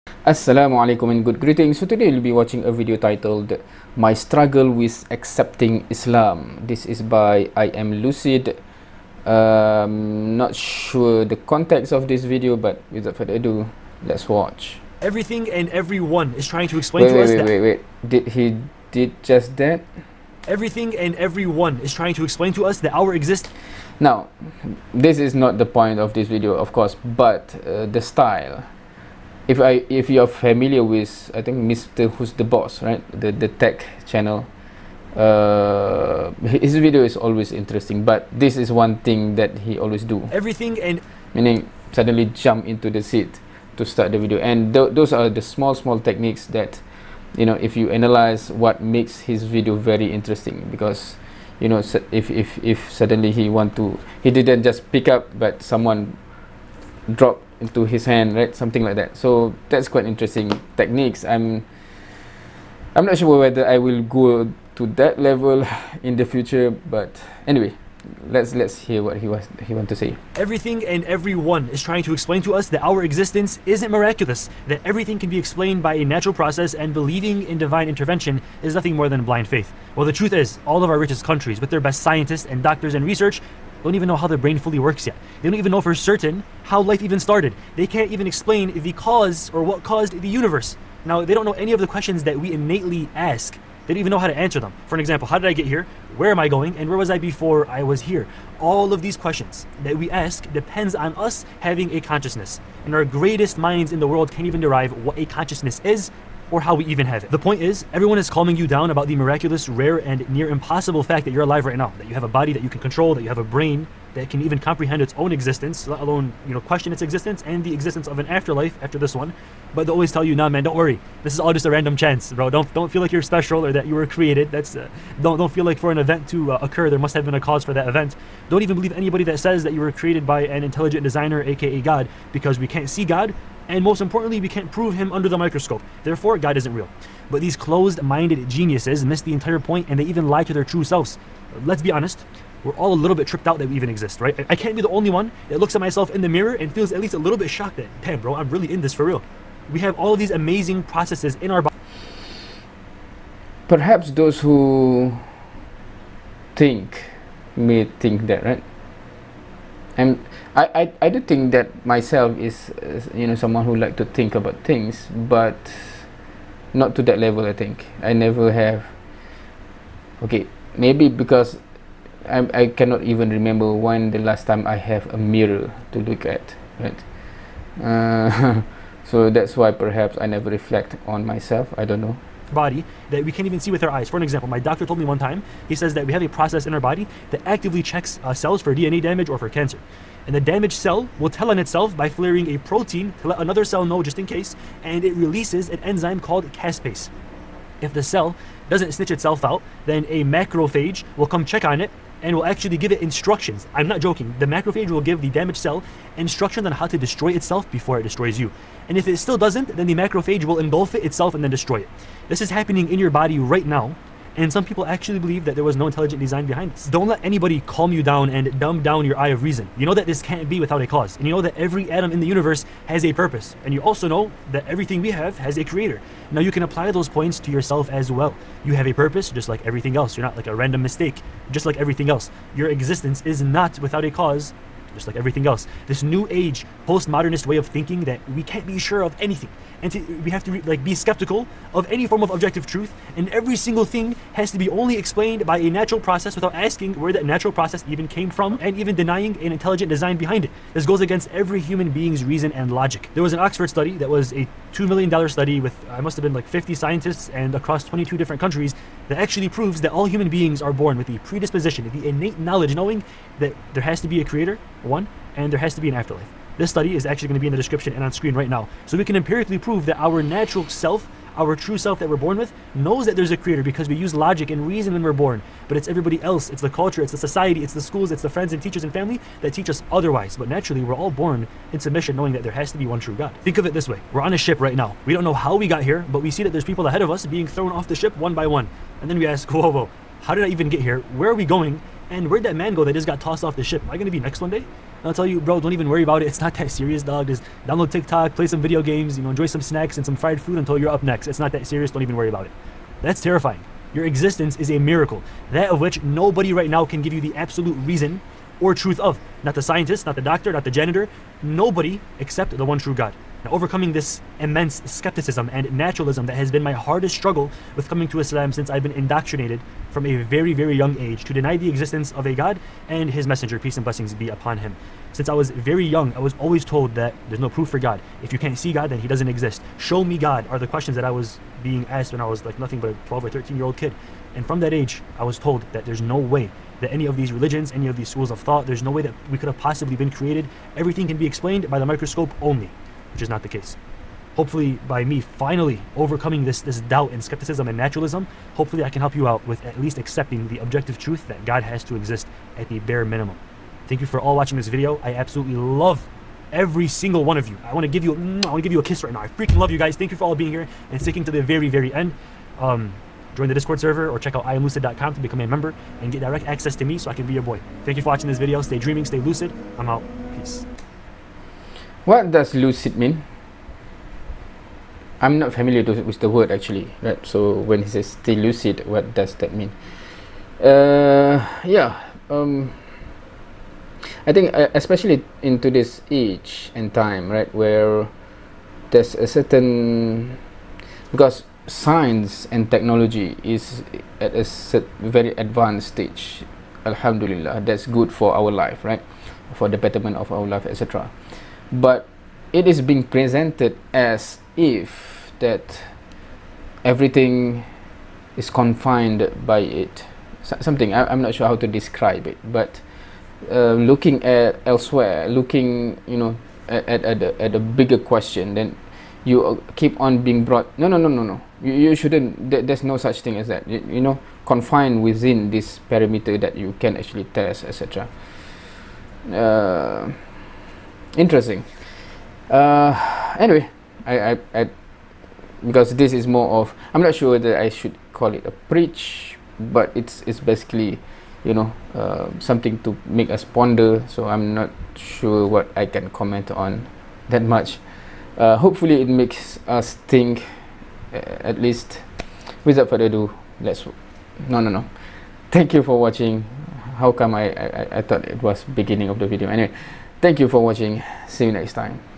My struggle with accepting Islam - A Muslim's Reaction
In this video I watch and react to a video by @iamLucid titled My struggle with accepting Islam

Interestingly I think most of what is highlighted here is more of the challenge with the mainstream worldview to accept God, or a creator